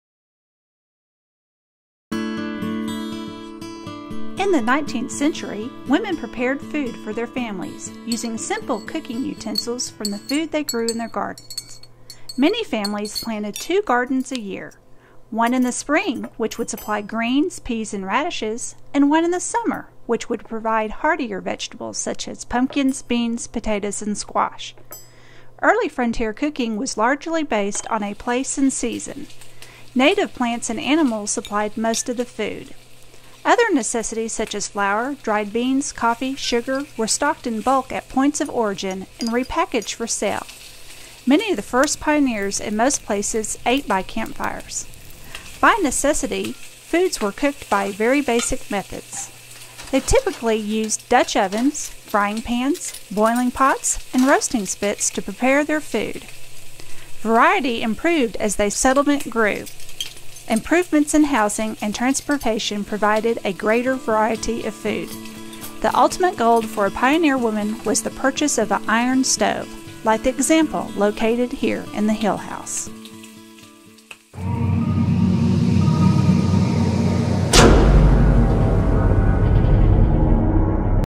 A03c: Cooking a Tasty Meal
This video is about Cooking a tasty meal on the Frontier, and how people ate different types of food in that period of time.